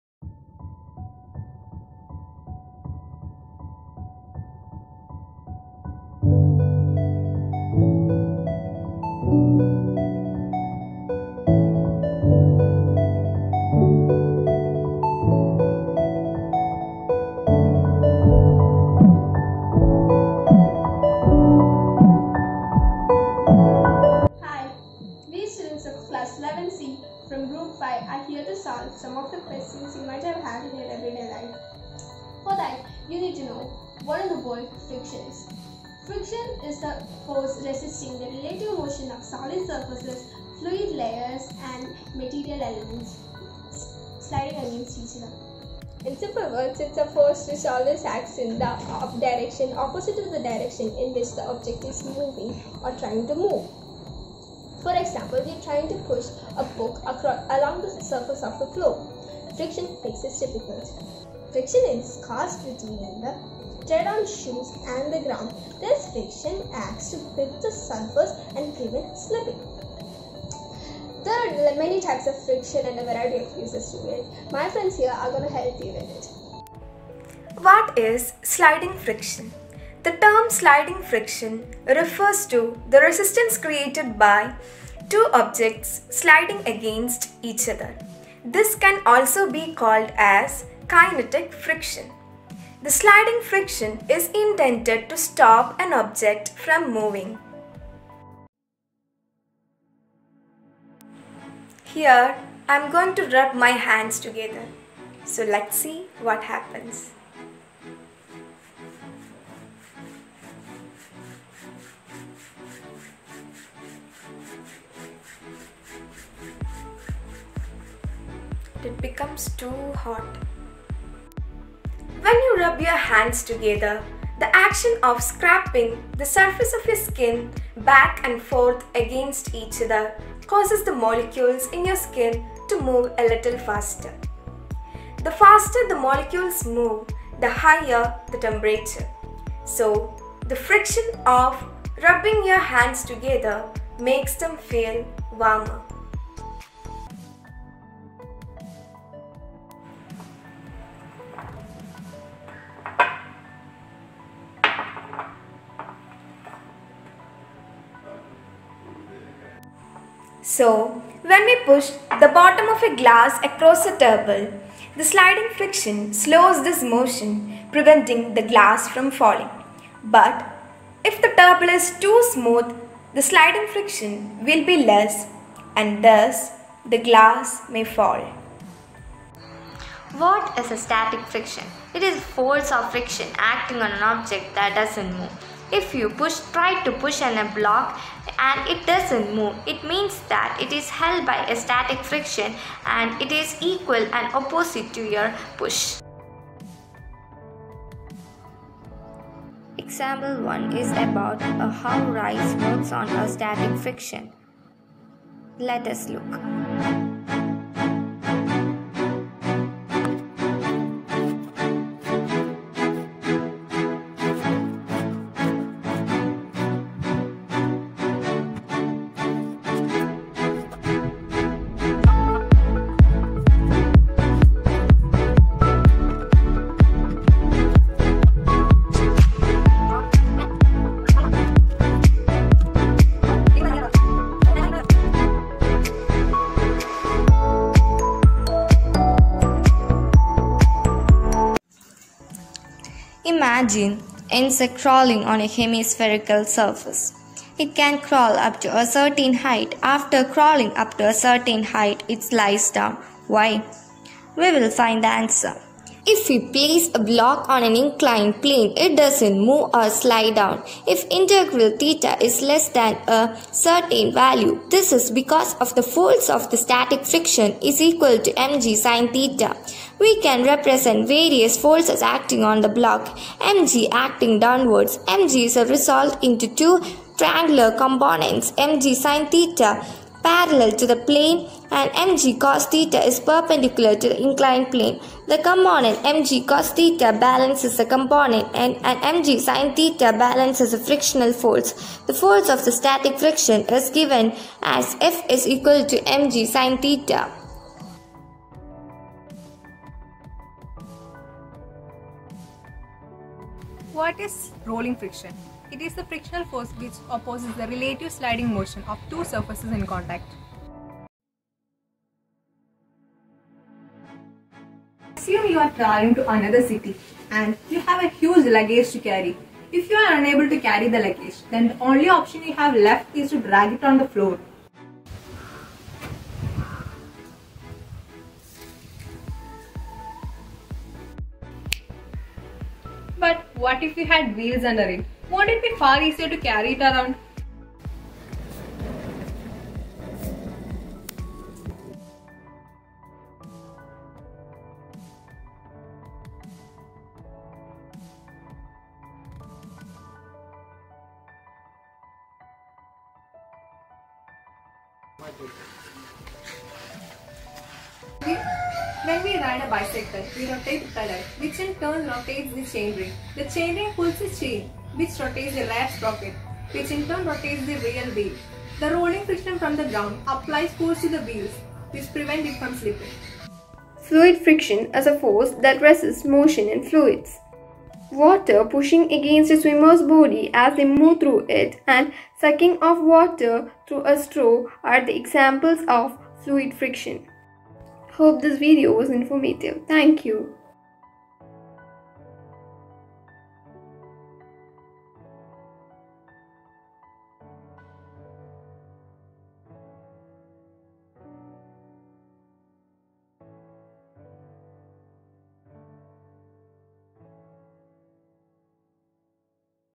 Art Integrated Project Physics |Class 11 S.N. Vidya Bhavan Chentrappinni
An Art Integrated Project done by students of 11th from S.N. Vidya Bhavan.
This is a Physics projected with the topic Friction.
We do not own the Animated Videos used in the video and the credits of it go to its owner.